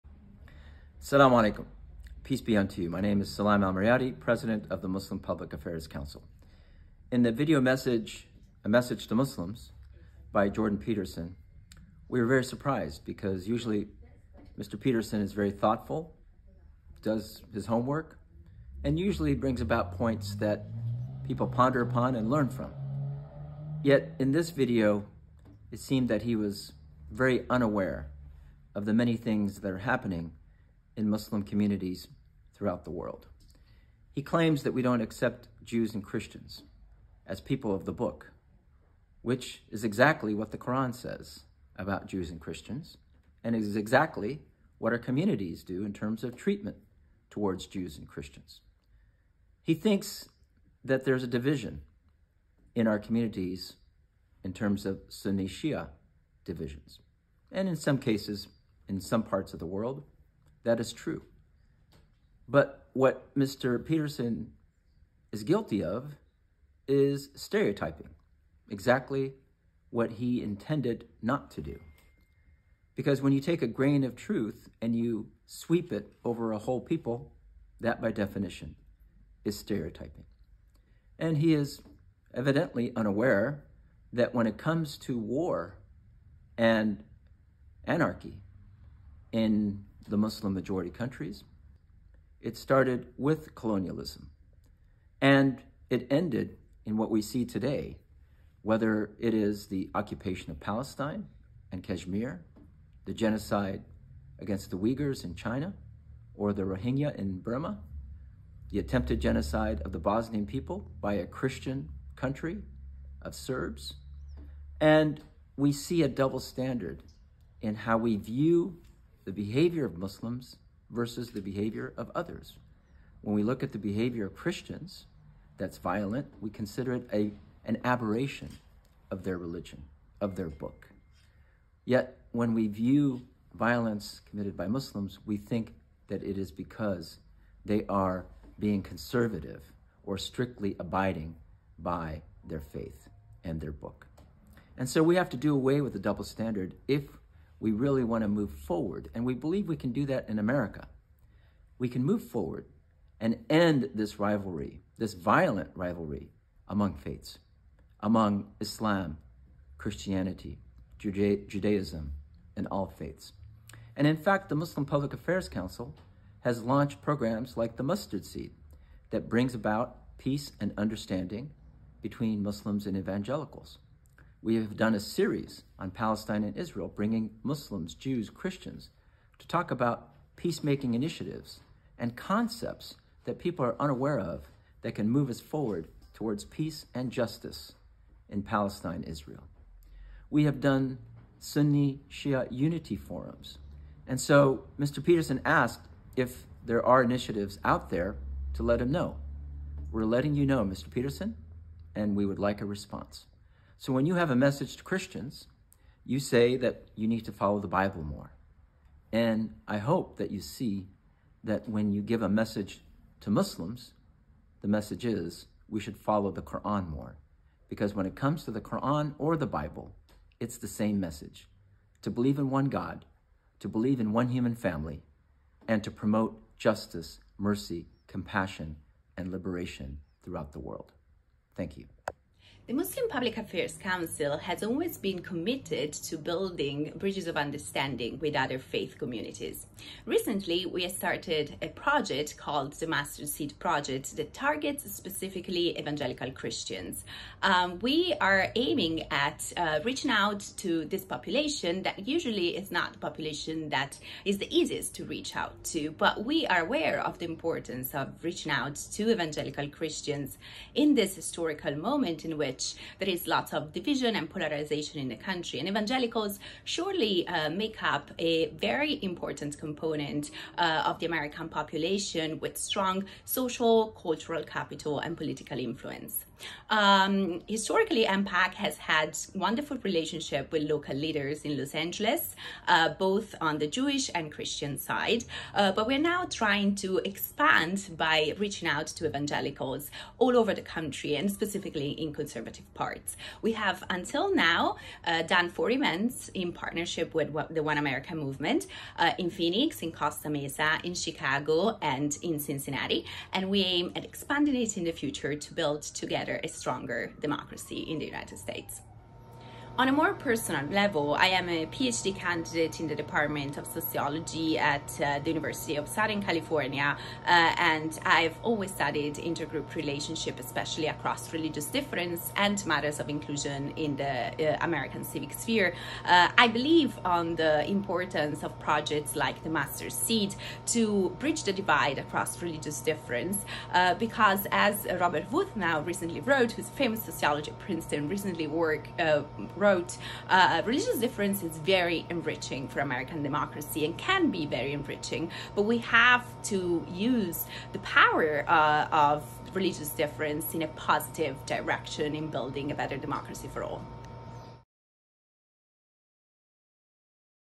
A Response to Dr. Jordan Peterson’s Comments
Recently, a YouTube video was posted disparaging our community, assuming and insisting that American Muslims are not working to further dialogue, both internally within our faith and externally.

Usually, we would not engage such an individual, one whose goal seems disingenuous given his background.

However, given that such a significant part of the policy work we do at the Muslim Public Affairs Council is done through collaboration with members of the faith community, we decided that a response was necessary.

That individual is Dr. Jordan Peterson, a social media influencer who works with Ben Shapiro's conservative news site, The Daily Wire.

Ensuring positive policy outcomes for American Muslims is at the core of our mission in Washington D.C.

To share our work in this critical collaboration, please hear more about our work below.